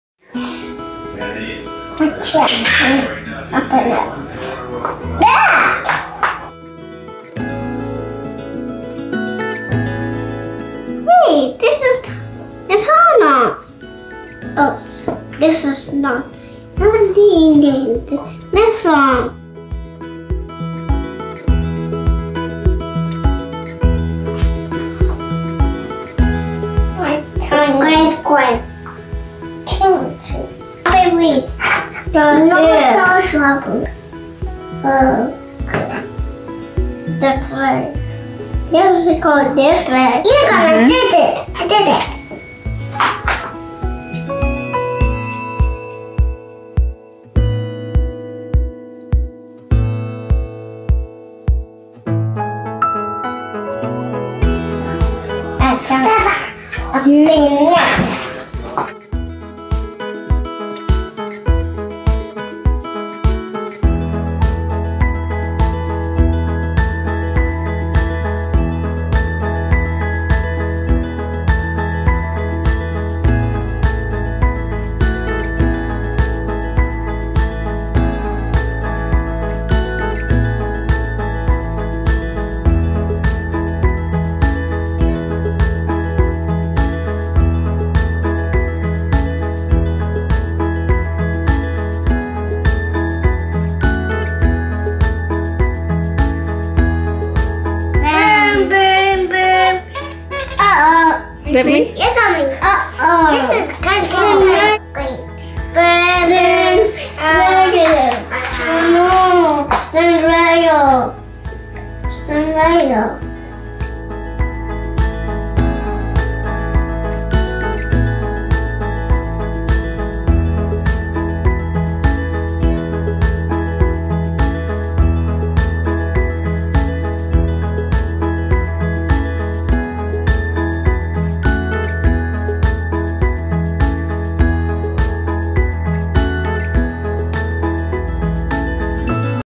Racecar and Caterpillar fruit snacks
Made with grapes,  apples and strawberries.
If these 2 year olds can do it so can you